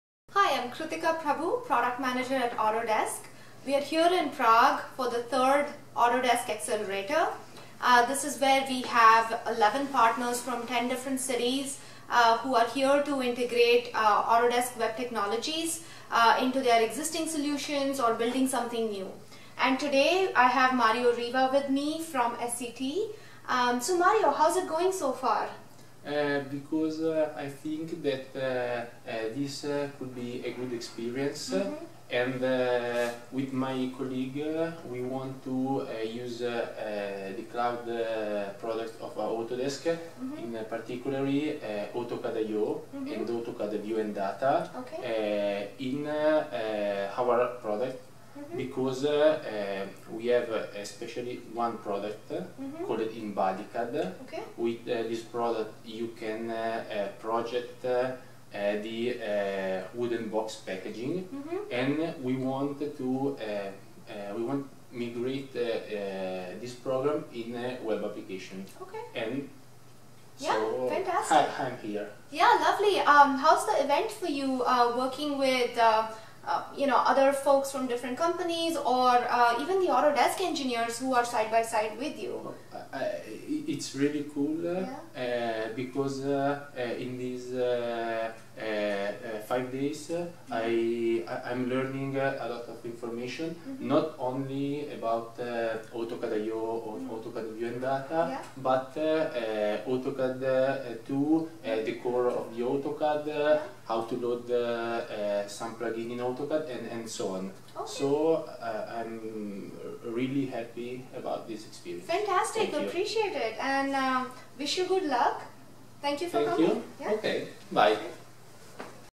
Hear from SCT on their experience at our Accelerator event in Prague.
Mario Riva from SCT, Italy, talks about his experience attending our Cloud Accelerator event in Prague, Czech Republic, September 2015